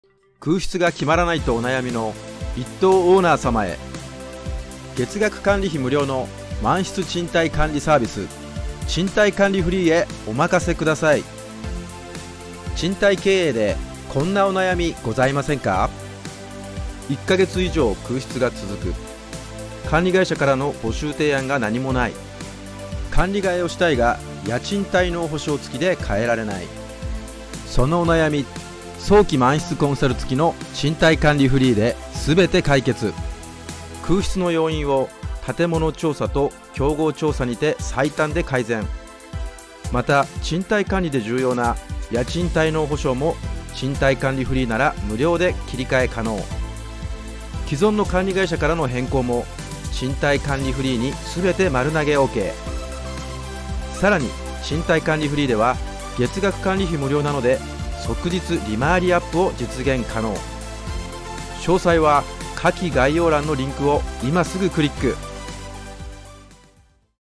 空室が決まらないと お悩みの一棟オーナー様へ「賃貸管理free!」へお任せ下さい。月額管理料0円で早期満室経営へ改善
「賃貸管理free!」は東京23区内のアパ・マンオーナー様へお勧めの月額管理費0円のこれからの賃貸管理のスタンダードです。利回りを改善したい・無気力な管理会社を変えたい・入居率にお悩みのオーナー様は今すぐ下記リンクをご確認ください！